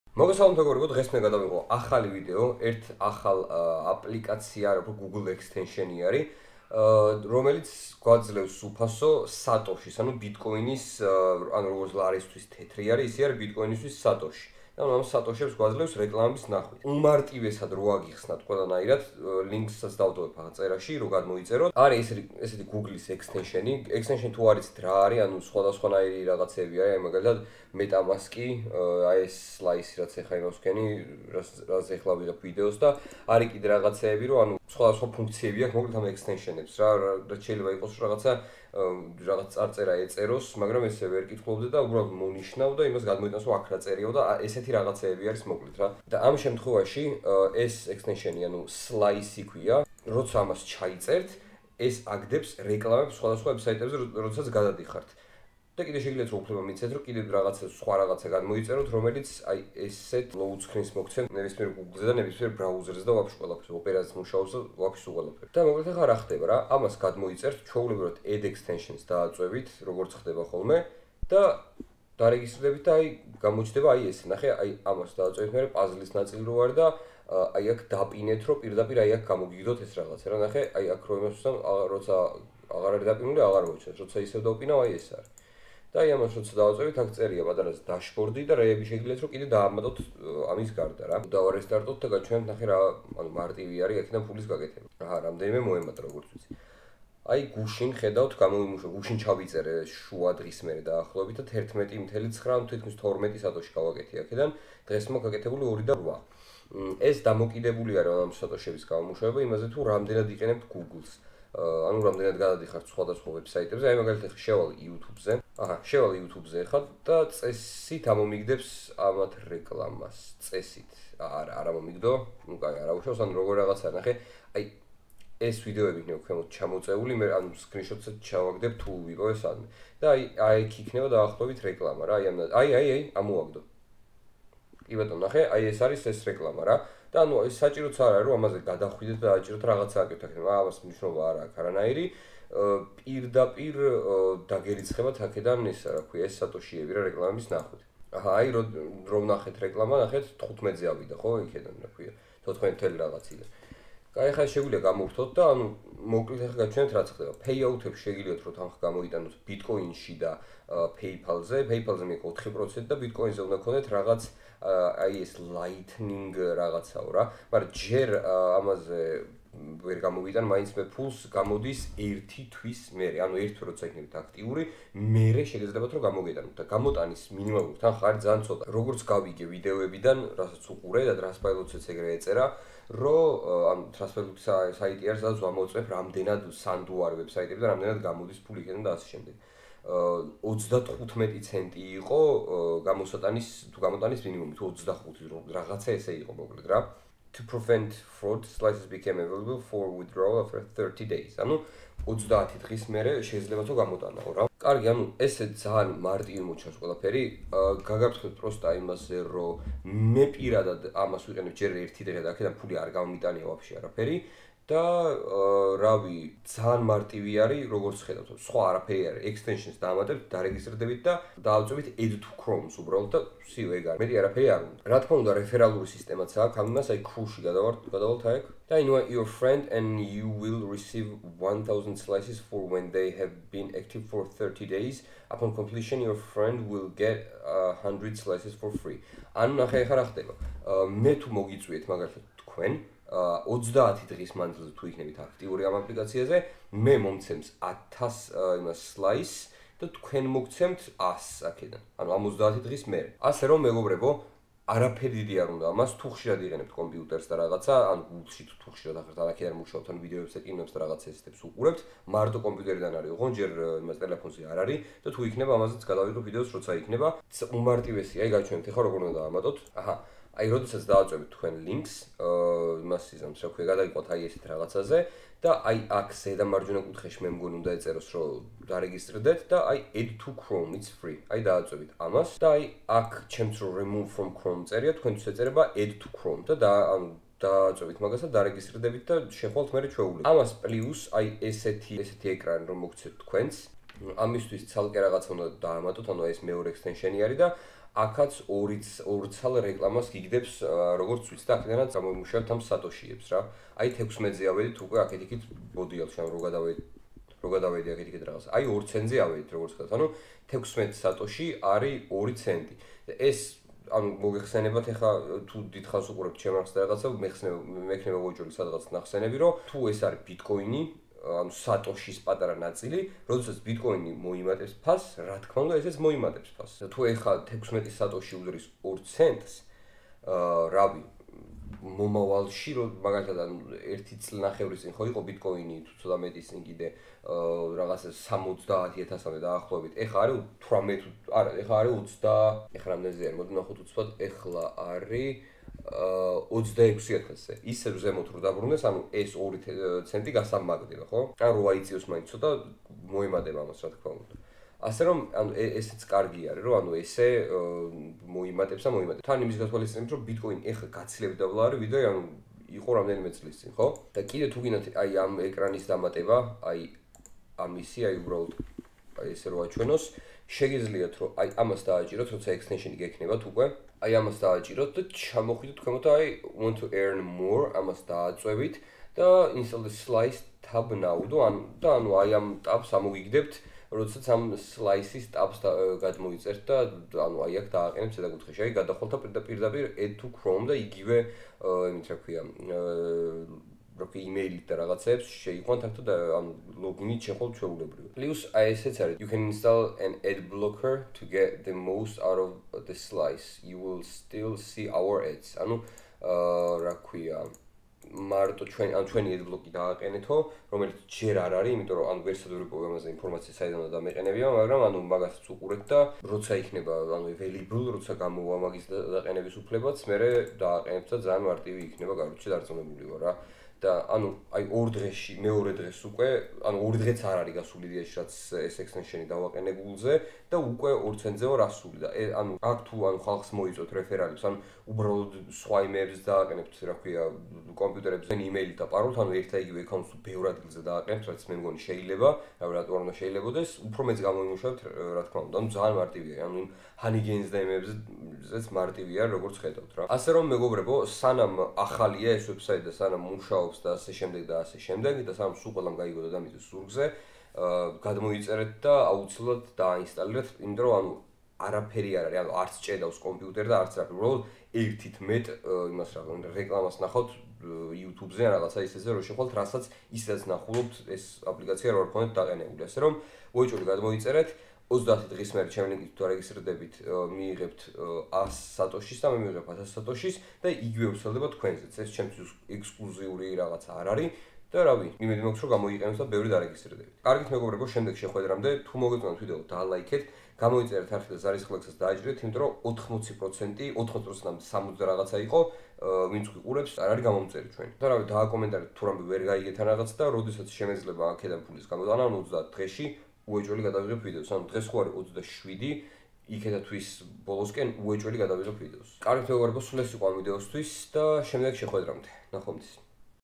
როგორ გავაკეთოთ ფული რეკლამების ყურებით უფასოდ!!! slice
Slice არის ახალი აპლიკაცია რომლის საშუალებითაც შეგვიძლია გავაკეთოთ ფული რეკლამების ნახვით რომლებსაც უბრალოდ ამოაგდებს როდესაც სხვადასხვა საიტებზე შევალთ *გუგლის ჩვეულებრივი გამოყენების დროს* რაც უფასოა!!! ასერომ არ გაუშვათ ეს შანსი ხელიდან და გადმოიწერეთ ეს გუგლის ექსტენშენი